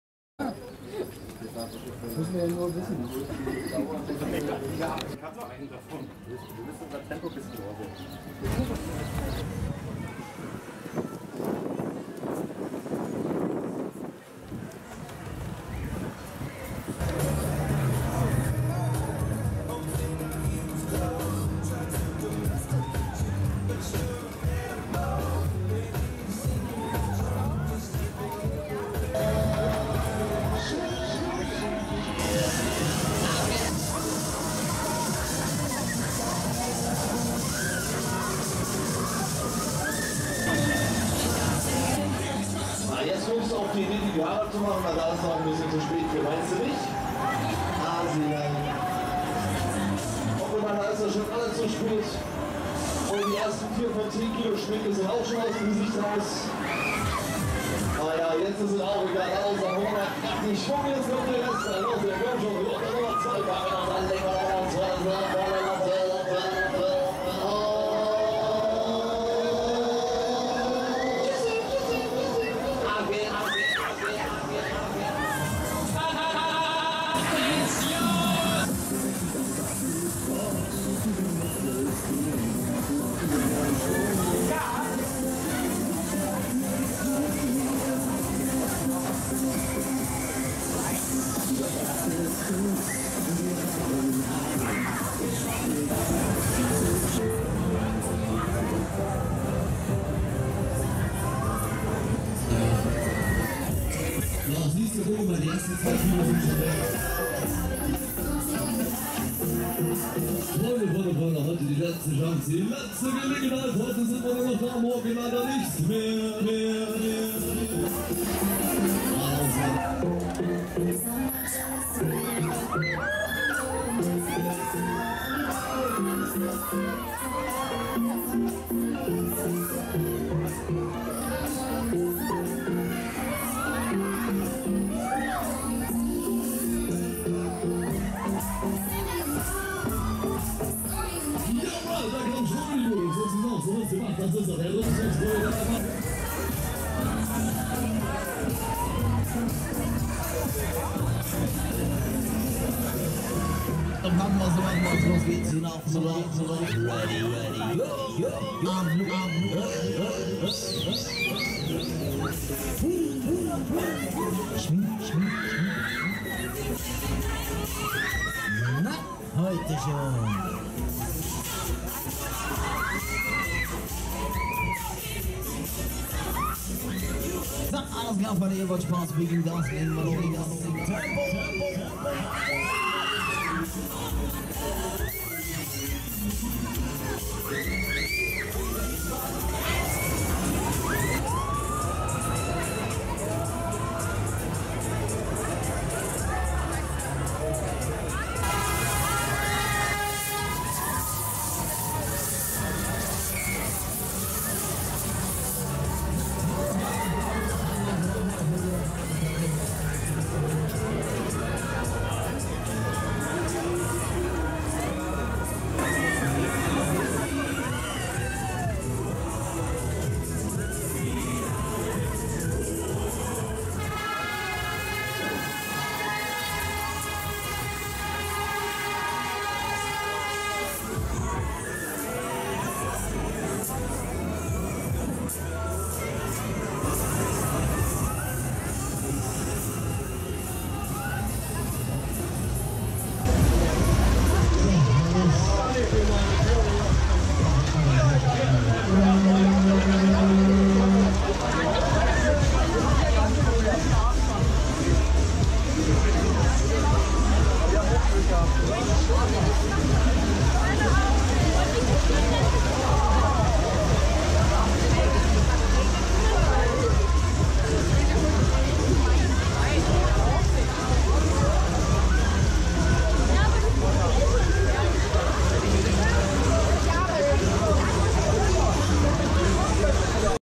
Hussitenfest Bernau 2018 // Fahrgeschäfte und Impressionen (Aftermovie)
sitenfest Bernau 2018 // Fahrgeschäfte und Impressionen (Aftermovie), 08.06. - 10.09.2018

Fahrgeschäfte und Attraktionen:
Break Dance - Friedrich
Der Polyp - Friedrich
Hyper X - Rausch
Kettenflieger - Noack
Autoscooter, Riesenrad und diverse Kinderfahrgeschäfte